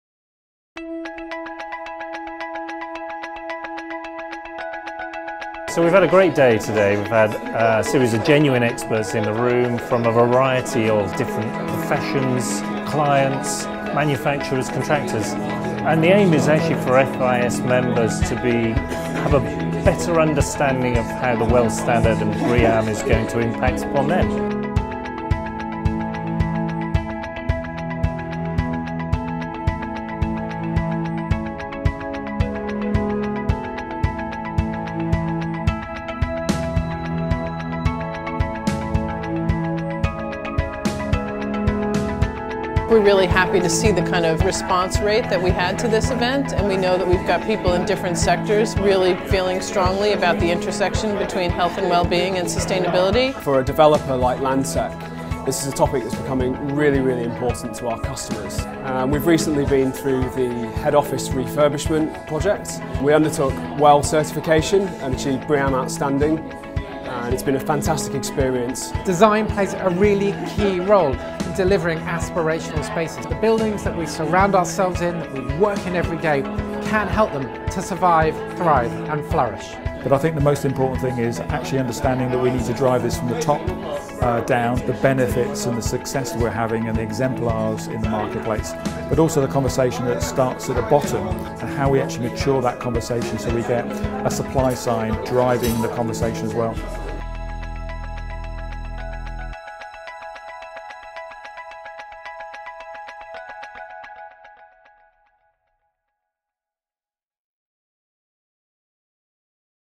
How we can integrate a human-centred approach to sustainability in buildings?
In October 2017, leading UK professionals from across real estate, architecture, design, sustainability and products met to help answer the question:  How we can integrate a human-centred approach to sustainability in the built environment? Hosted by FIS, in collaboration with IWBI and BRE, participants discussed the importance of health and well-being of building occupants alongside environmental sustainability, looking at future challenges, misconceptions and economic issues.